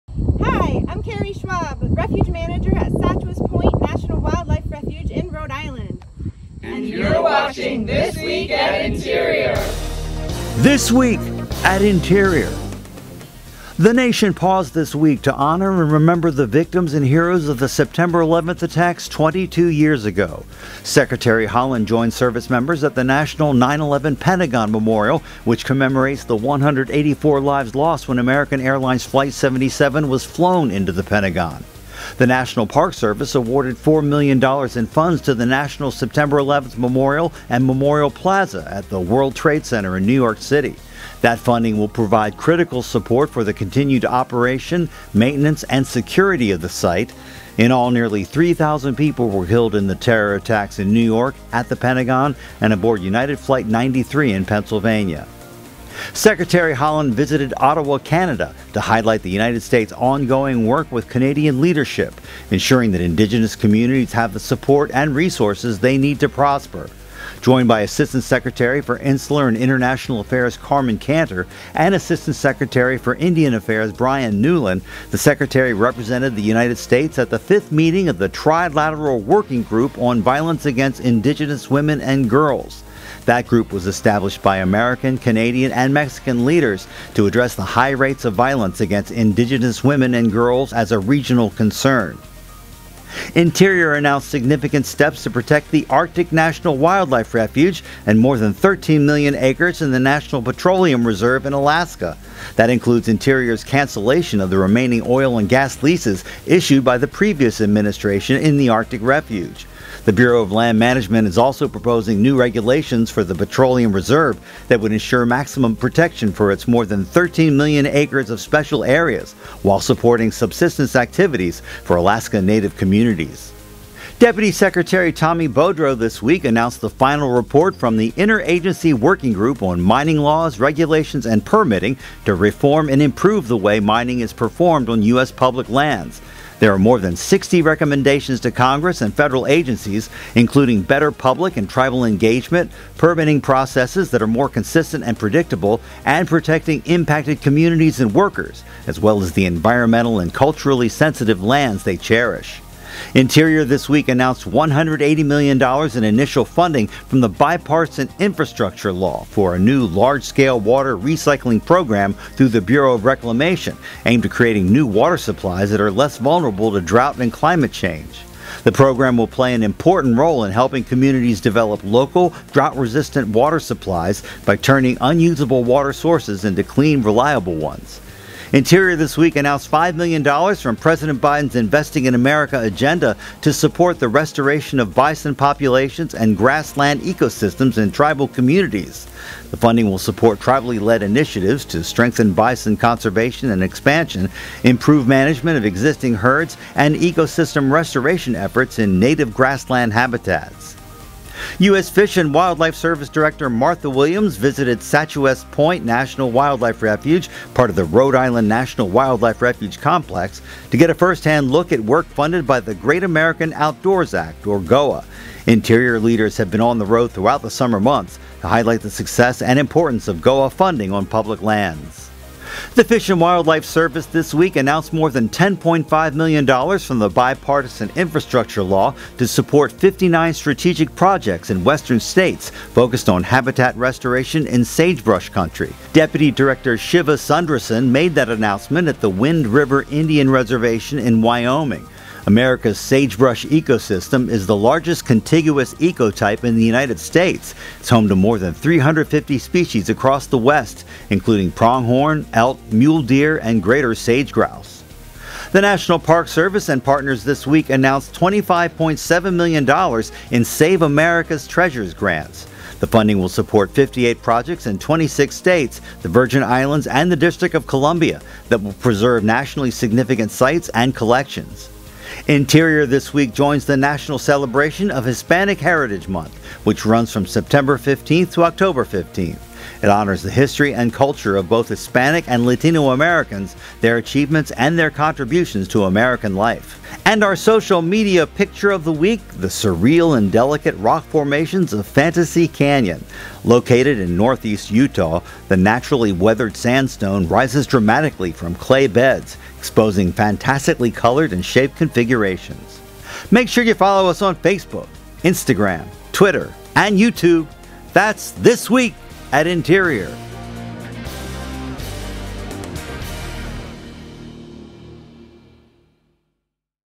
This Week at Interior September 15, 2023
This Week: Remembering the heroes and victims of the September 11th attacks; Secretary Haaland visits Canada to highlight work to support Indigenous communities; Interior announces significant steps to protect the Arctic National Wildlife Refuge and more than 13 million acres in the National Petroleum Reserve in Alaska; a new report makes recommendations for mining reform; a new large-scale water recycling program aims to create new water supplies that are less vulnerable to drought and climate change; Interior announces $5 million for bison and grassland restoration; the Great American Outdoors Act continues to have a positive impact on our public lands; the U.S. Fish and Wildlife Service announces $10.5 million from the President’s Investing in America agenda for habitat restoration in sagebrush country; there are new grants to Save America's Treasures; Interior celebrates Hispanic Heritage Month; and we head for the Utah desert in our social media Picture of the Week!